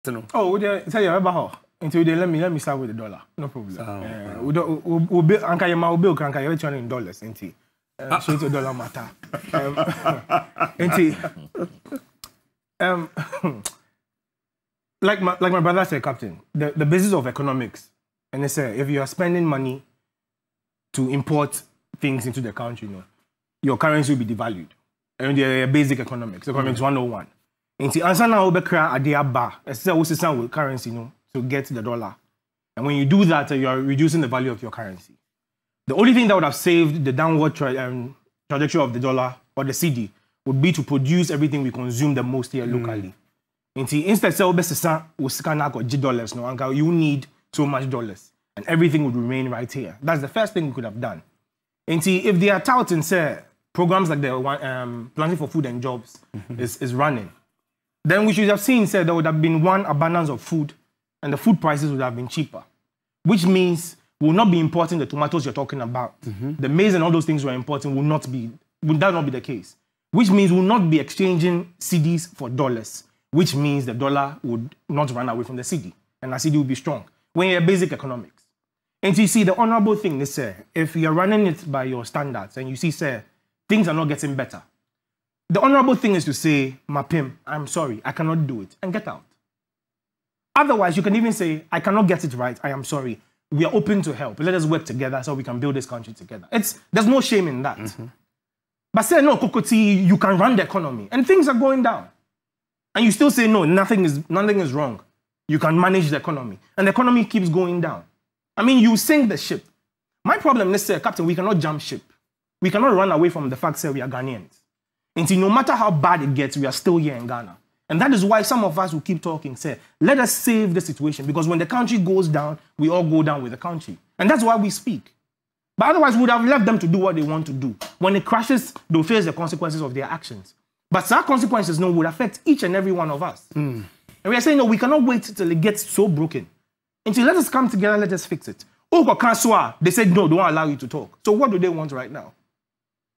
Don't be arrogant and accept your failure to stabilize the currency - Nii Aryee
Don't be arrogant and accept your failure to stabilize the currency - Nii Aryee on the economic Guru's inaction in response to the depreciation of the Cedi